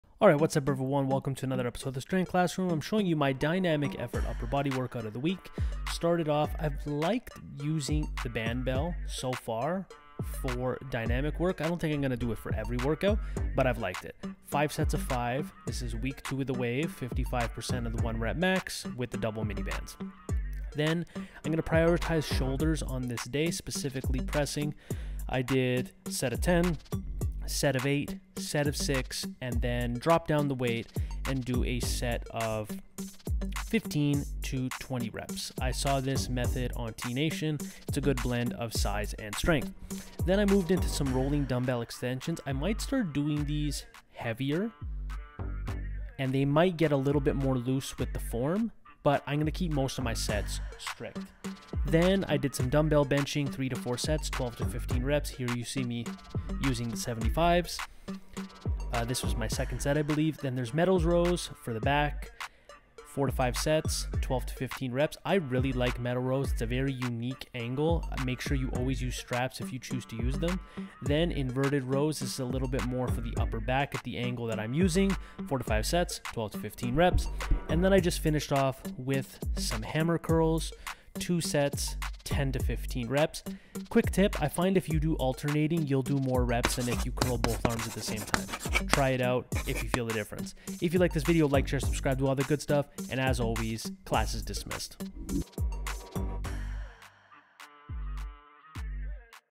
VLOG: Dynamic BENCH on Incline
Sometimes you need to do your speed work from another angle other than flat bench.